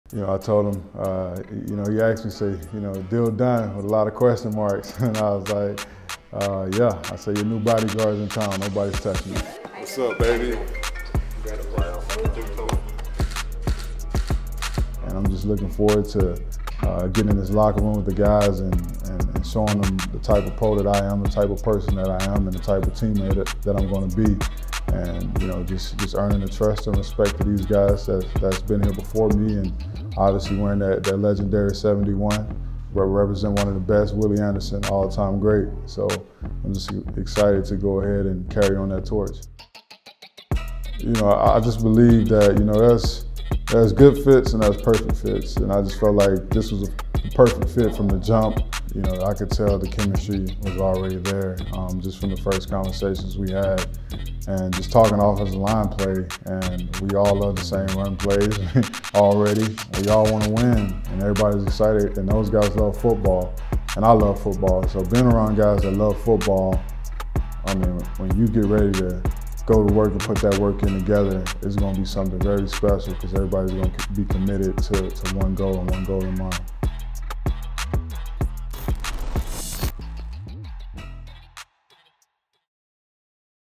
Meet La'el Collins | Cincinnati Bengals
Get to know the newest member of the Cincinnati Bengals, La'el Collins.